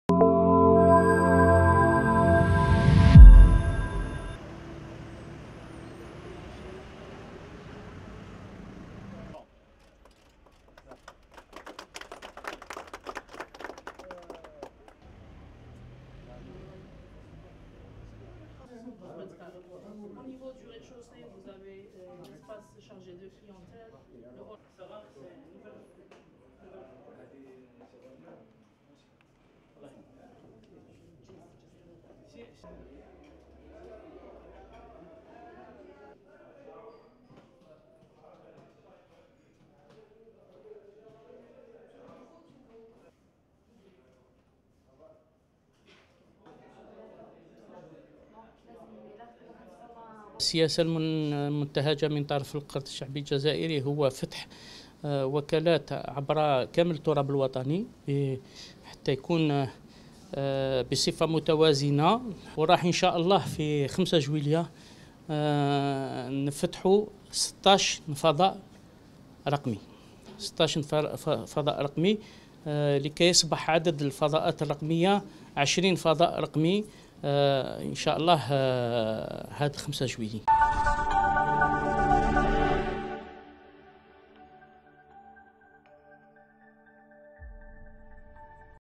تيزي وزو: افتتاح وكالة تجارية جديدة للقرض الشعبي الجزائري بعزازقة
تيزي وزو (وأج) - افتتح القرض الشعبي الجزائري, وكالة بنكية جديدة بعزازقة (تيزي وزو), تنفيذا لاستراتيجيته الرامية إلى التقرب من زبائنه ومرافقة الفاعلين الاقتصاديين المحليين.